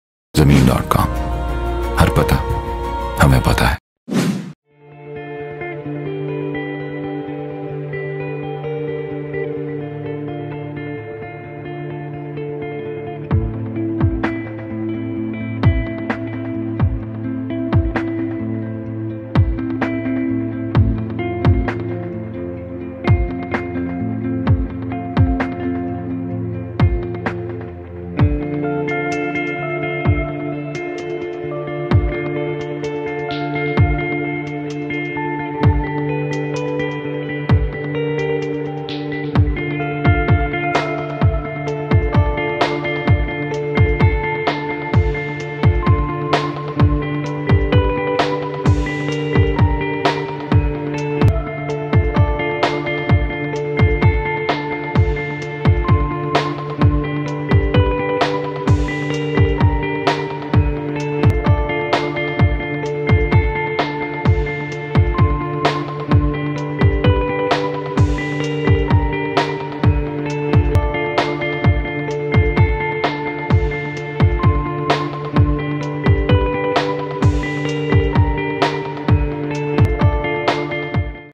6 MARLA OFFICE FOR SALE IN AL HAFEEZ HEIGHTS GULBERG LAHORE
Office is located at 7th. floor of Al-Hafeez Heights
(65-D-1, Sir Syed Road/Ghalib Road, Liberty Commercial zone Gulberg 3, Lahore).
Office is rented to a reputable Software Company at rent Rs. 104500. Rent payment is on quarterly basis.

Plaza has direct approach from Main Boulevard and M. M Alam Road. It is located in the vicinity of Hafeez Center and Liberty Market Gulberg.
Plaza has four lifts with back-up Generator. Back-up Generator Facility is also available. It has two Bathrooms and one kitchen.